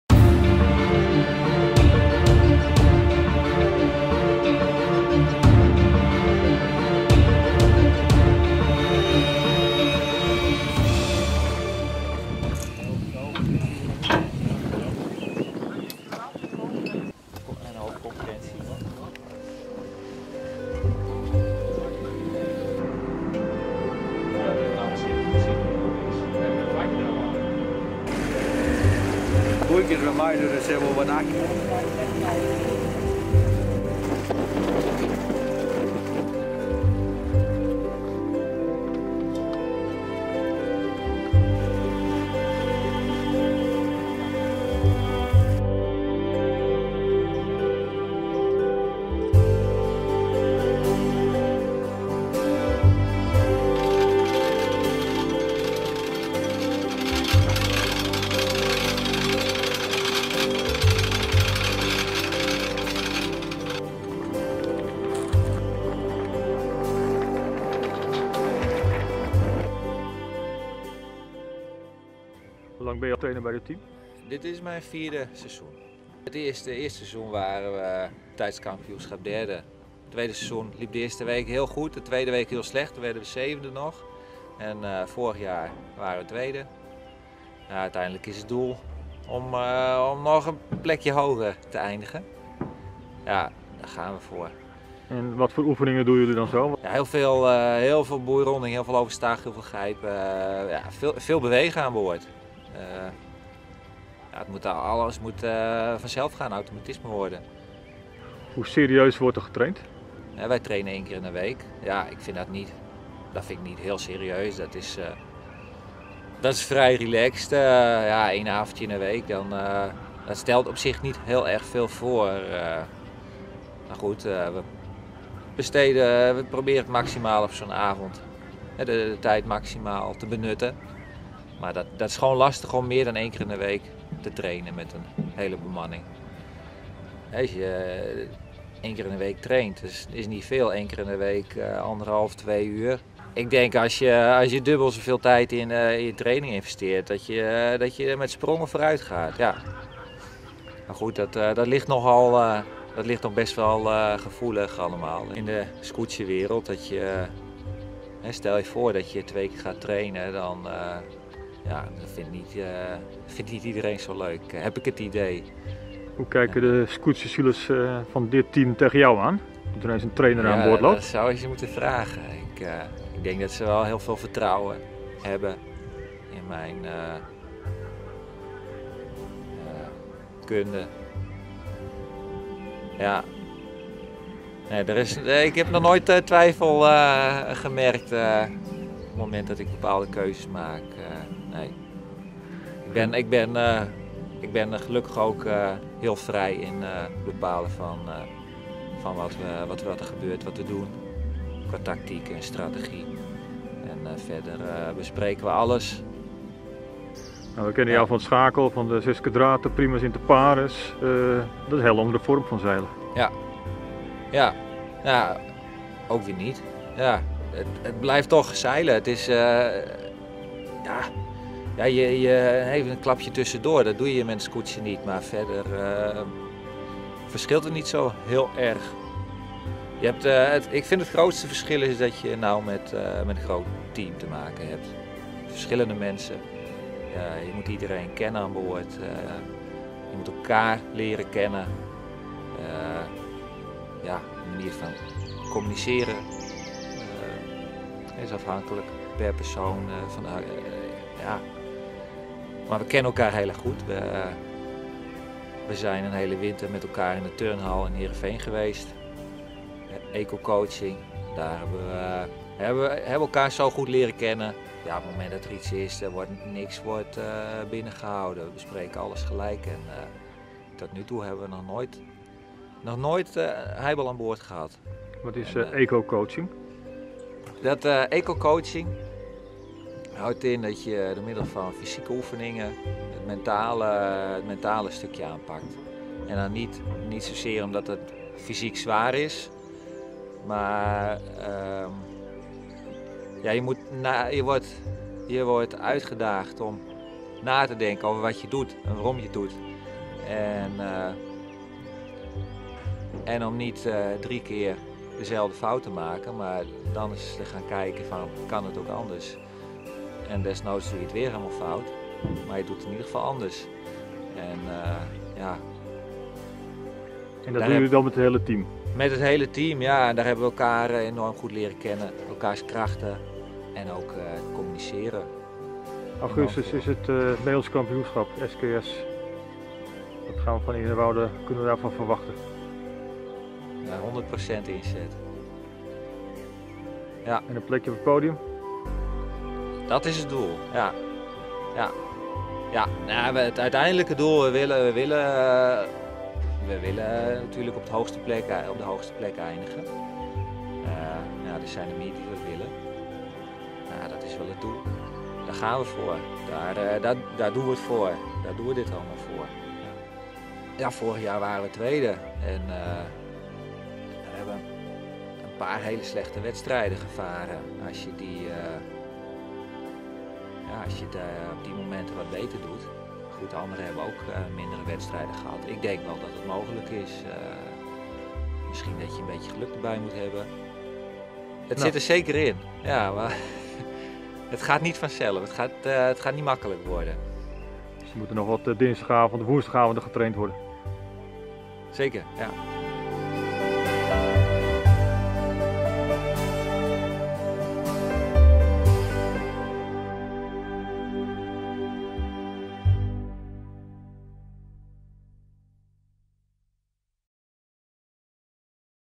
Skutsje Eernewoude alles voor de titel
Het team van het skutsje van Eernewoude werd afgelopen jaar tweede tijdens het SKS-kampioenschap. Het winnen van de titel is dit jaar het doel. In de aanloop naar het kampioenschap wordt iedere woensdagavond op het Sneekermeer getraind. Dit gebeurt onder leiding van meervoudig Nederlands kampioen in de Schakel en 16m2 klasse, Thijs Kort. In deze mini-documentaire volgt Watersport-TV een trainingsavond.